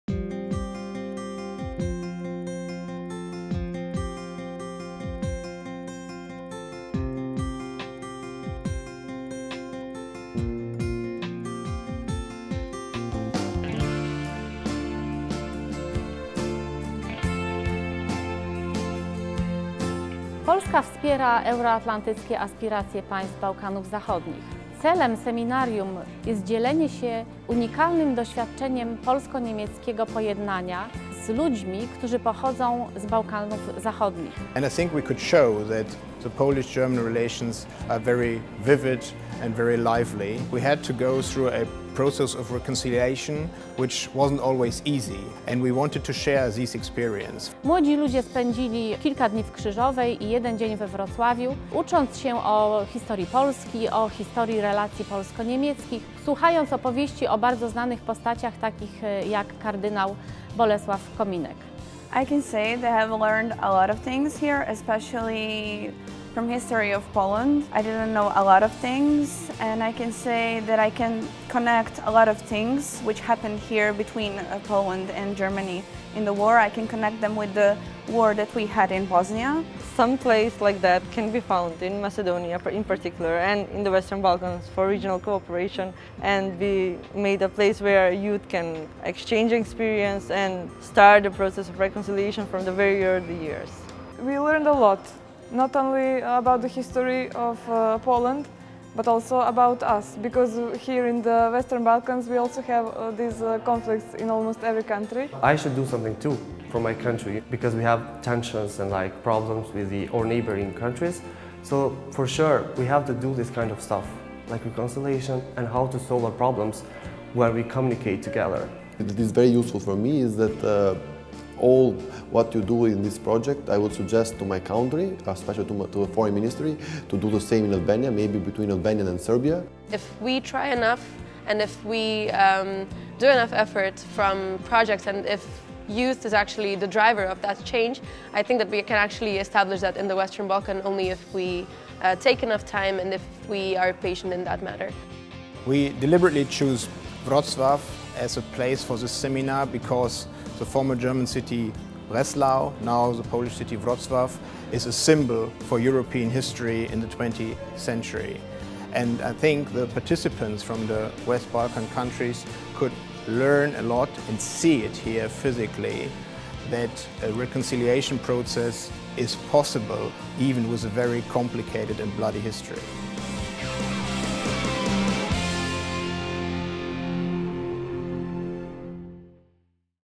„Pojednanie i pamięć” - seminarium dla młodzieży z Bałkanów Zachodnich
Studenci z państw Bałkanów Zachodnich wzięli udział w pierwszym seminarium organizowanym w Krzyżowej i Wrocławiu przez Ministerstwo Spraw Zagranicznych we współpracy z Ambasadą Republiki Federalnej Niemiec oraz Fundacją „Krzyżowa” dla Porozumienia Europejskiego.